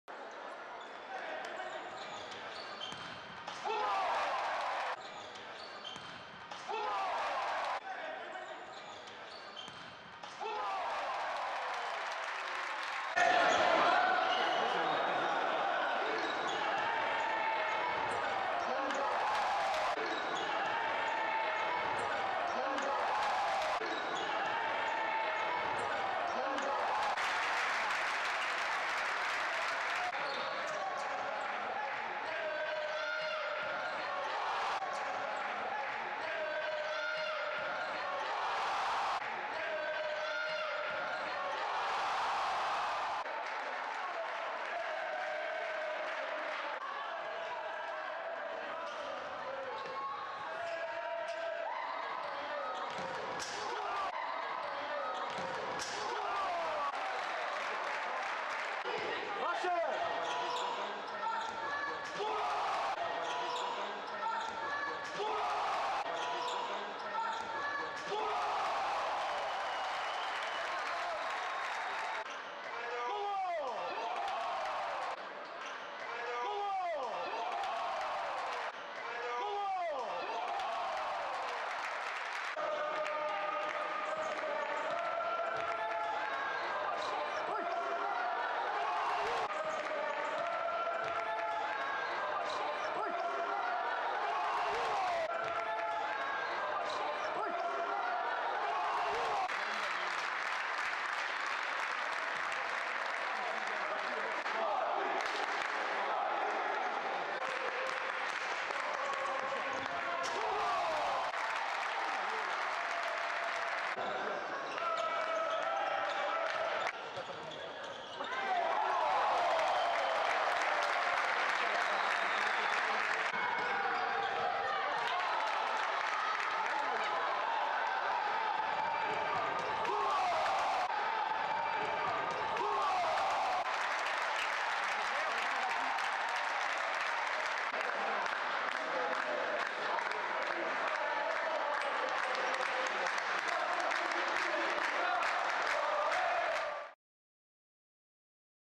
Liga Placard: Belenenses 1 - 10 Sporting CP
O Sporting venceu, por 10-1, na deslocação ao reduto do CF Os Belenenses, no jogo inaugural da segunda jornada da Liga Placard.

Consulta a nossa programação em: canal11.pt